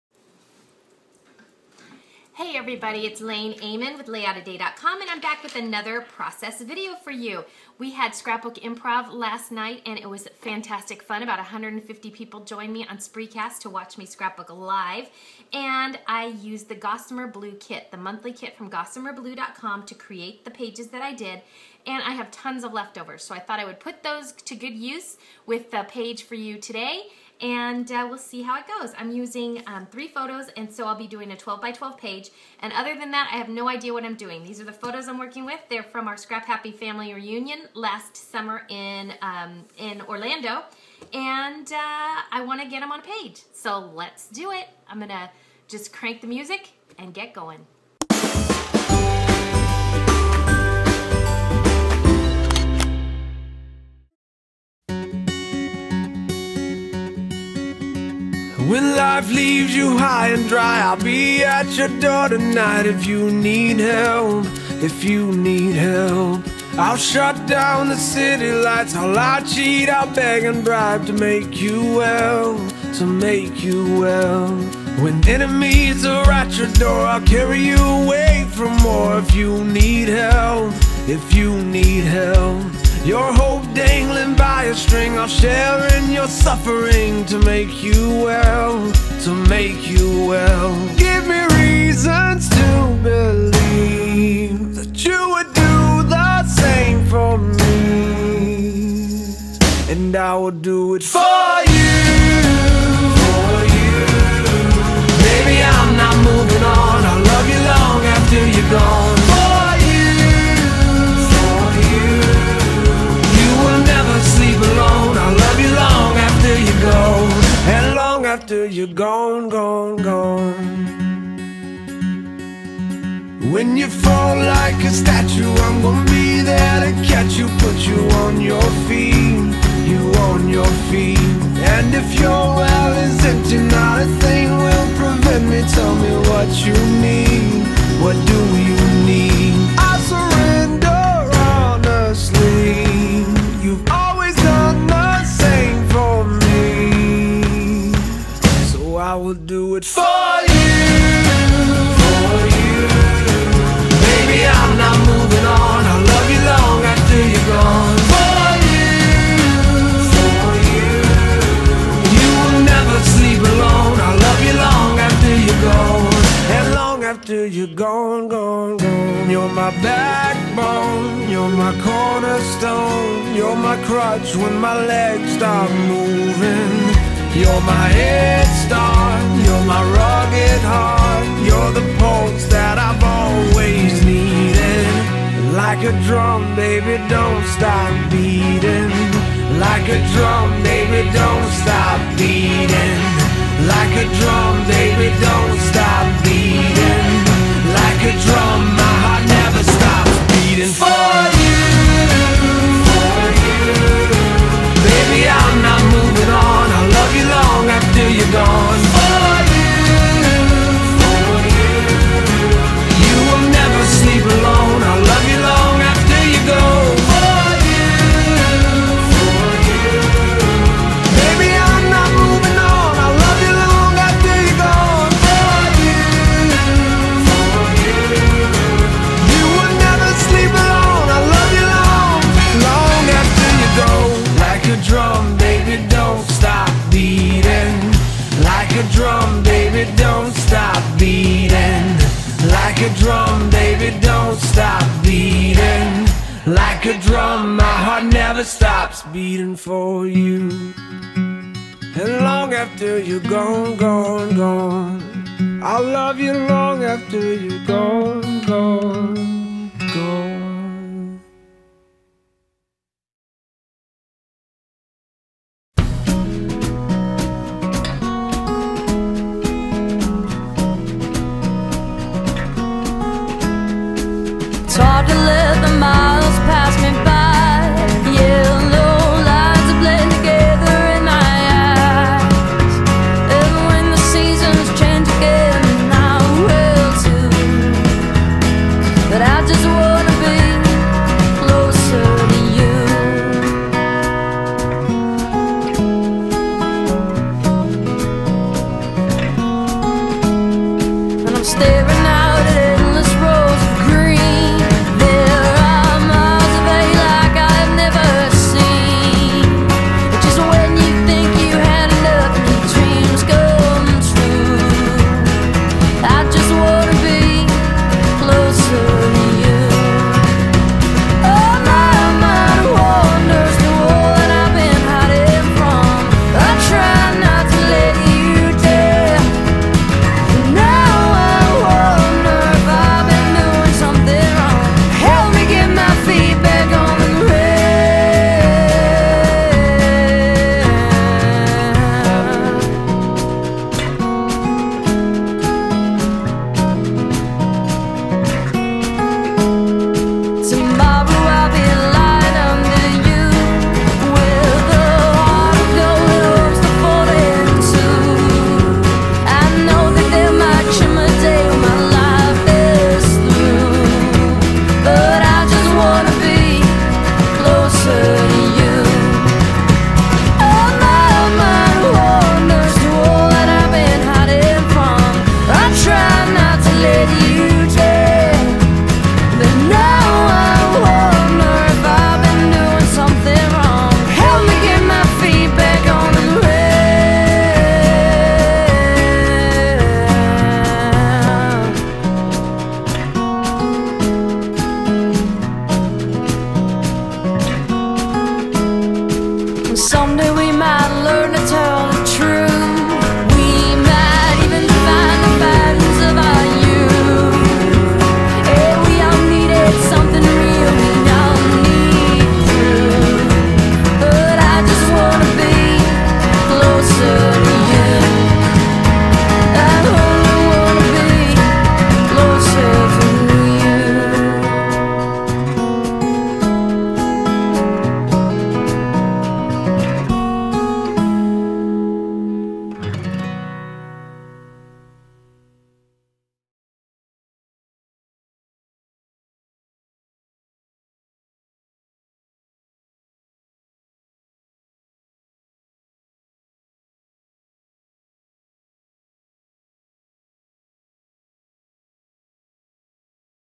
"Scrapbooking Process Video" #2 by @lainehmann of Layoutaday.com.
I'm back with another quick and easy scrapbooking layout process video! This one features leftovers from the Gossamer Blue May 2013 kit. If you're looking for scrapbooking ideas and inspiration with some peppy music, watch me create this 12x12 layout with two photos. The actual layout took about 30 minutes to create, but the sped up version is less than 7. :)
For more tips for simple scrapbookers like YOU, please visit me Ideas and resources galore!